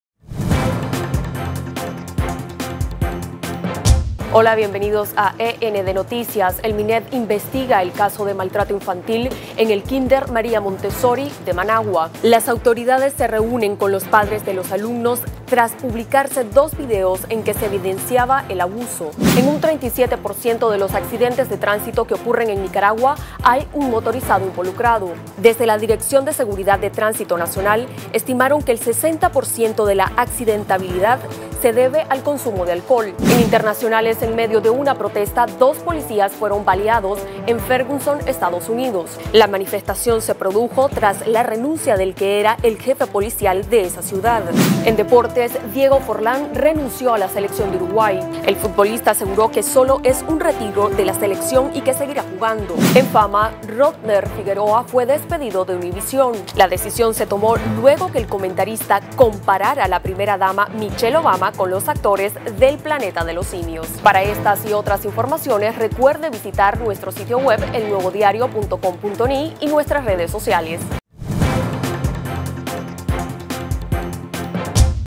END Noticias: Investigan maltrato infantil en Managua
En nuestro micronoticiero de hoy: Diego Forlán deja la selección de Uruguay, Rodner Figueroa fue despedido de Univisión. Esto y más en 60 segundos.